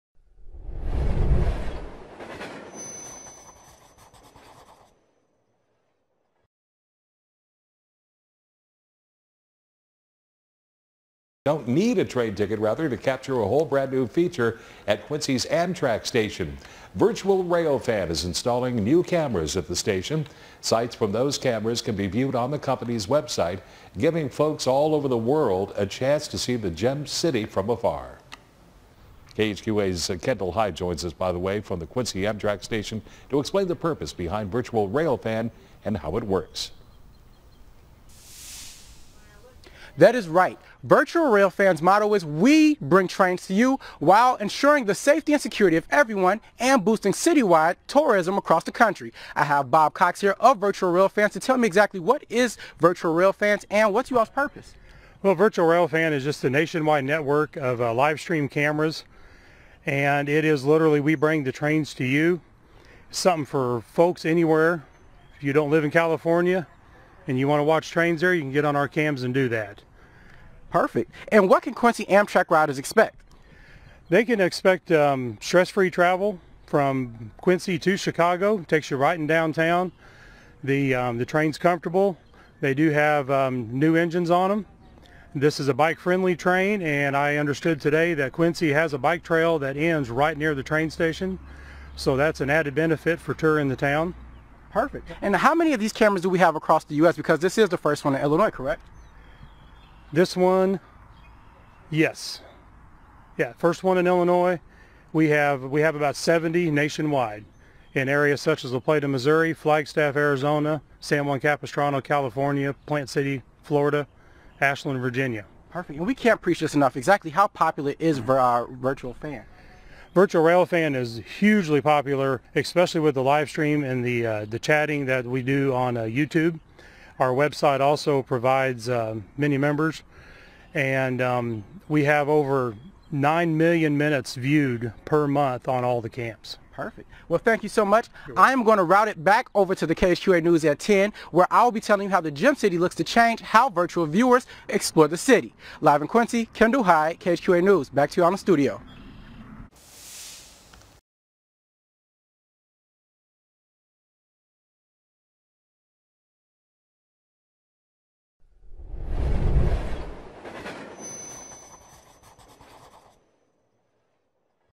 NEW CAMERA INSTALL!!! Bob Cox on Channel 7 KHQA, Quincy, IL representing Virtual Railfan.
Thanks Bob (Station Master at La Plata, Mo), you made us proud!  If all goes well we should be up and running by the end of this week.

Our sincere thanks to Channel 7, KHQA Quincy, IL for letting their viewers know about Virtual Railfan and our great members!